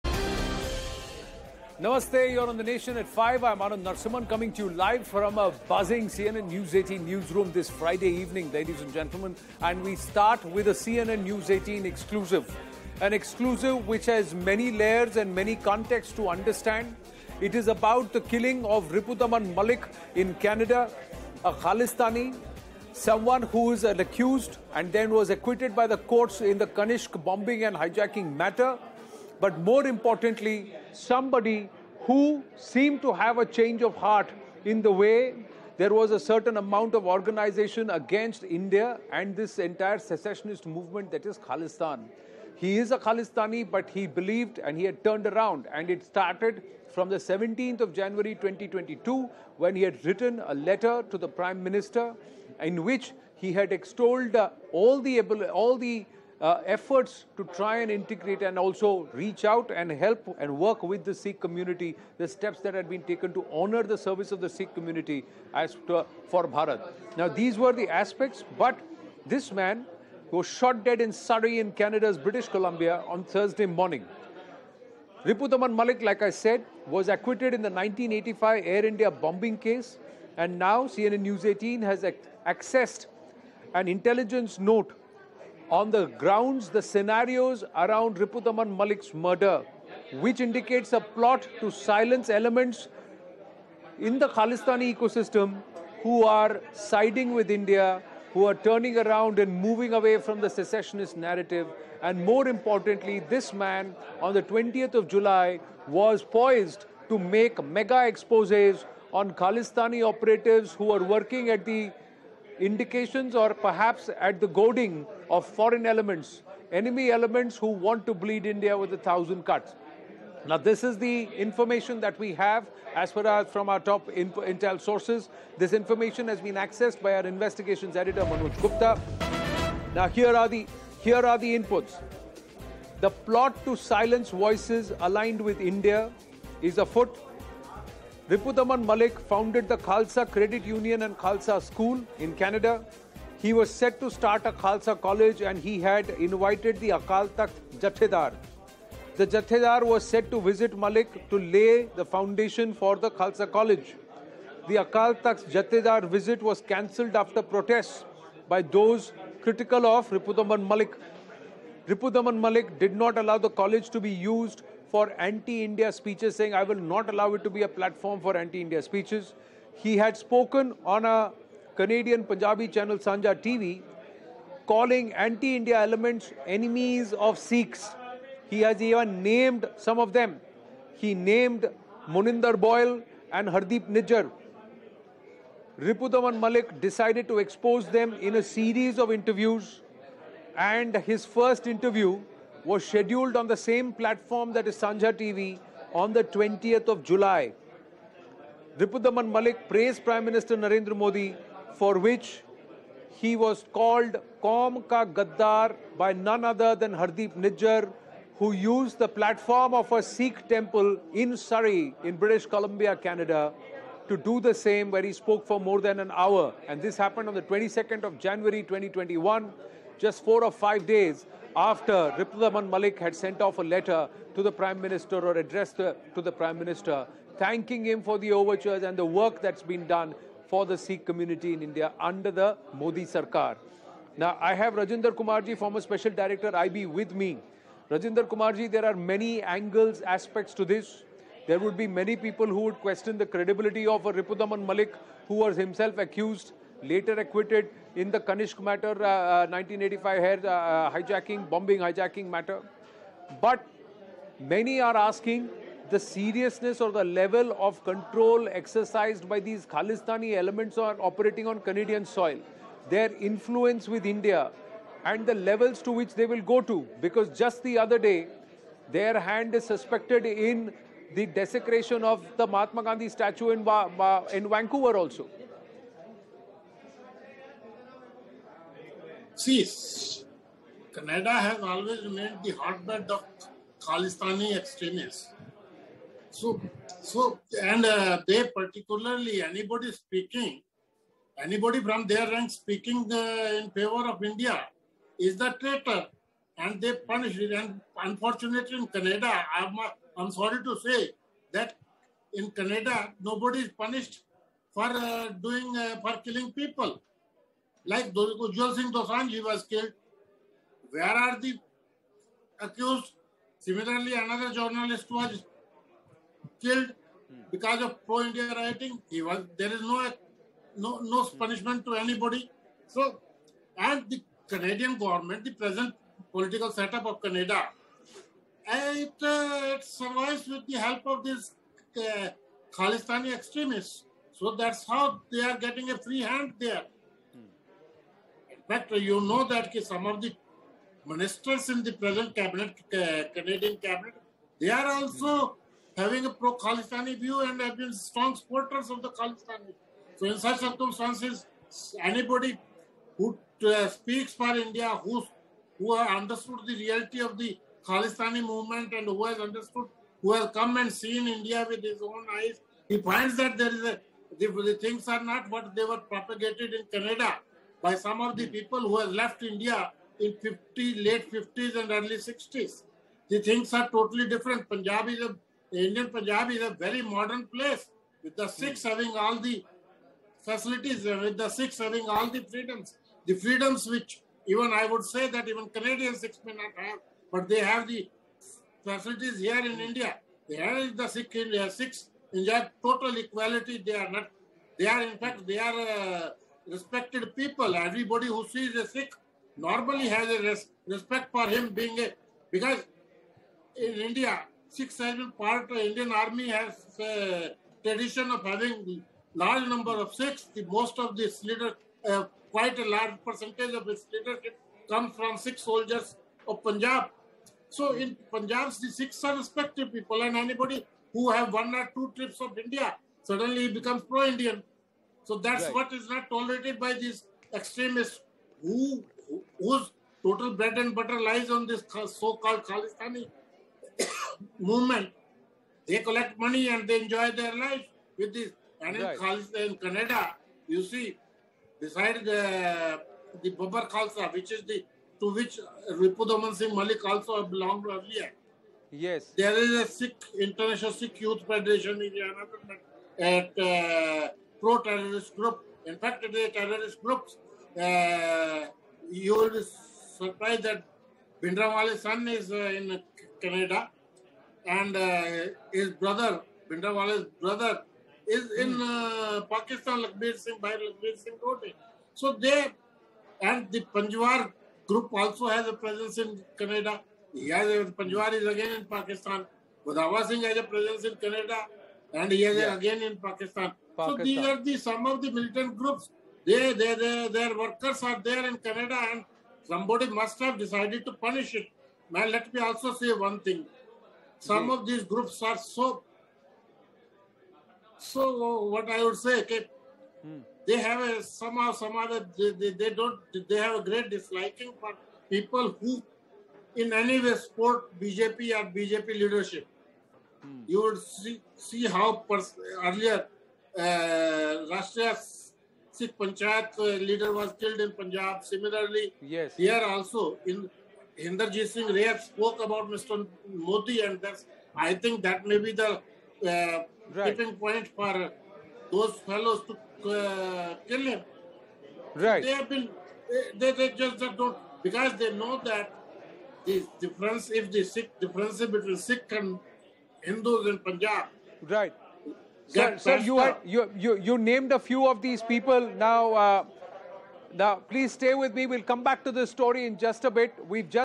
Was 1985 Air India Bombing Accused Ripudaman Malik Murdered For Backing India? | English News
Ripudaman Singh Malik, who was acquitted in the 1985 Air India bombing case, was shot dead in Surrey in Canada’s British Columbia on Thursday morning. According to an exclusive intelligence report, ISI is behind the murder of  Ripudaman Singh Malik. It is also reported that his ; 'pro-India stance' led to his killing. Former Special Director IB Rajinder Kumar shares his views. Listen in.